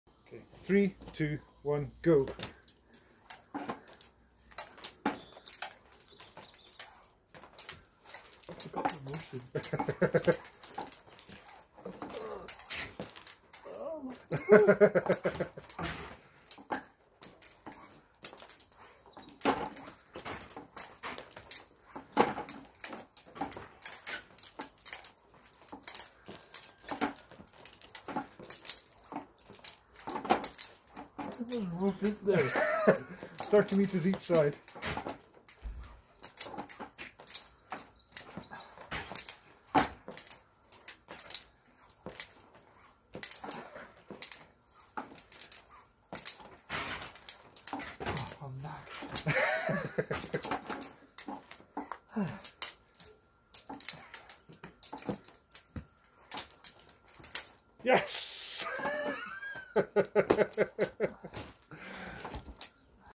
Lead belay fun learning
A fun belay competition to help teach lead belaying 🤣